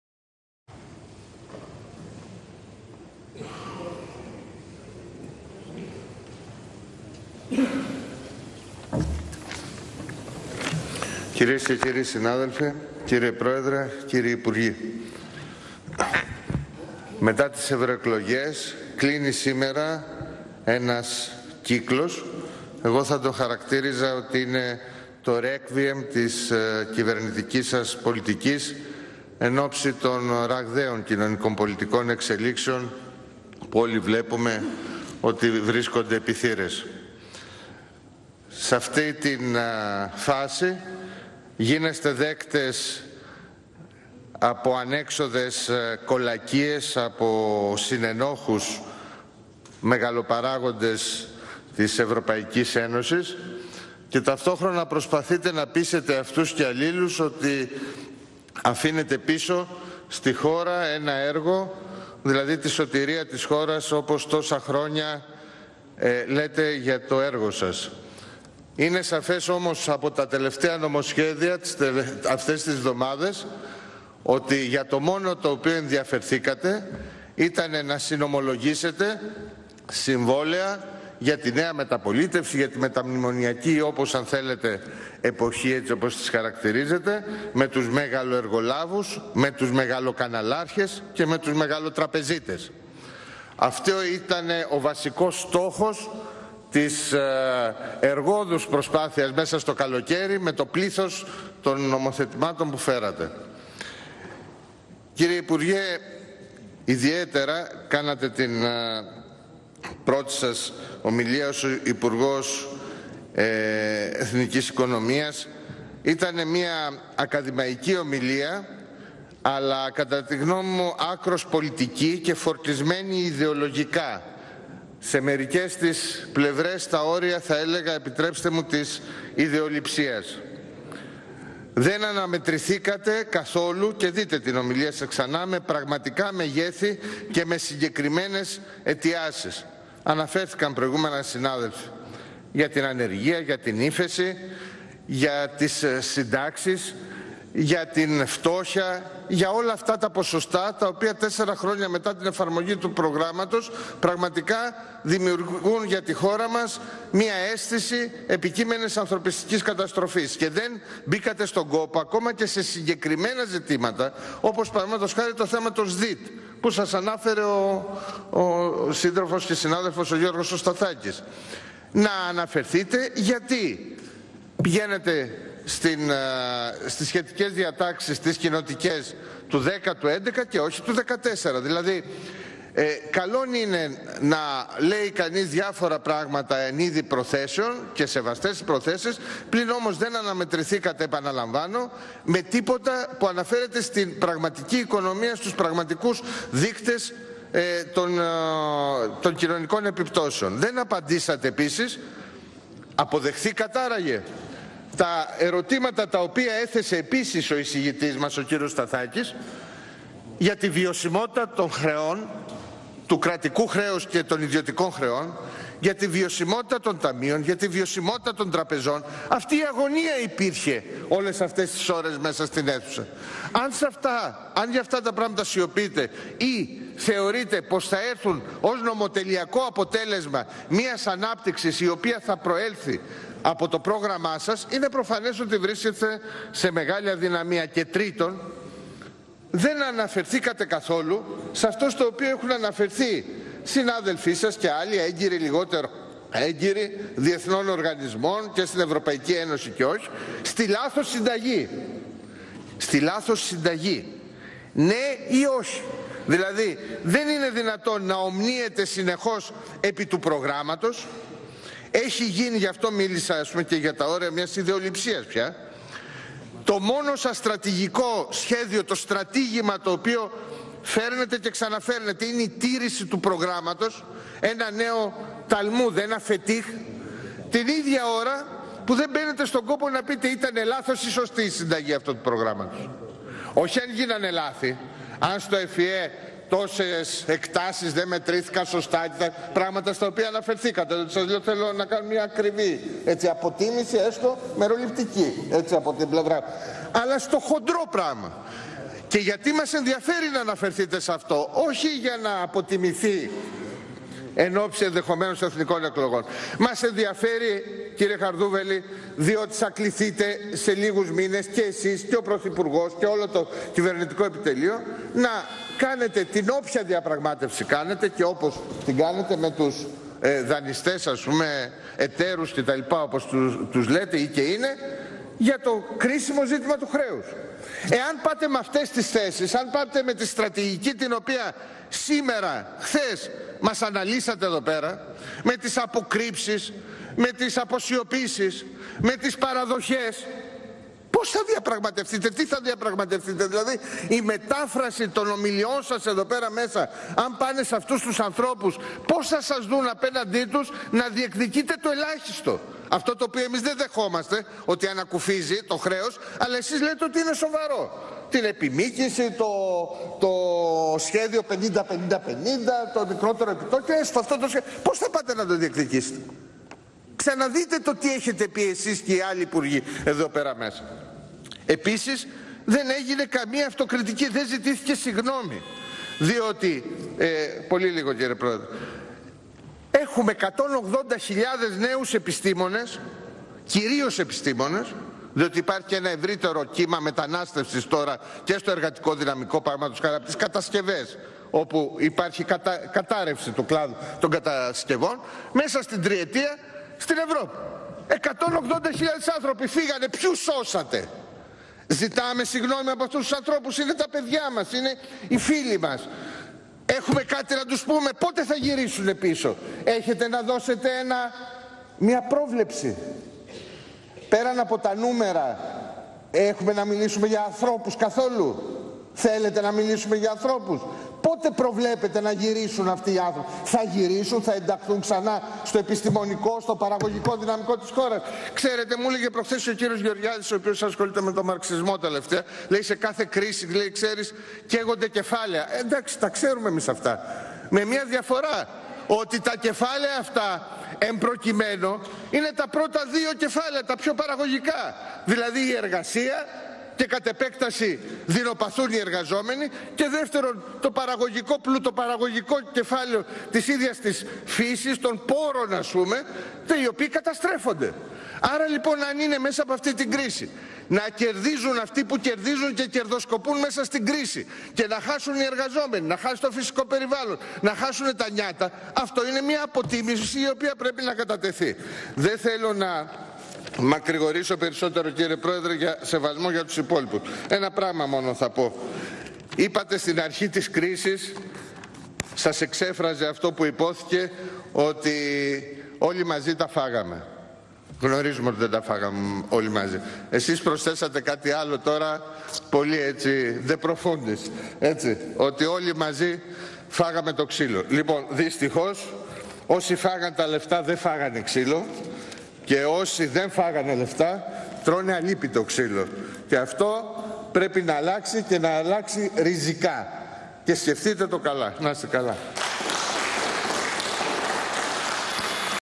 Ν. Βούτσης: Δυστυχώς όσοι δεν φάγαν τα λεφτά φάγαν αλύπητο ξύλο
Ομιλία Νίκου Βούτση κατά τη συζήτηση και ψήφιση του σ/ν του Υπ. Οικονομικών «Μέτρα στήριξης και ανάπτυξης της ελληνικής οικονομίας στο πλαίσιο εφαρμογής του ν. 41046/2012, οργανωτικά θέματα Υπουργείου Οικονομικών και άλλες διατάξεις», Ολομέλεια Β' Θερινού Τμήματος της Βουλής, 6-8-2014.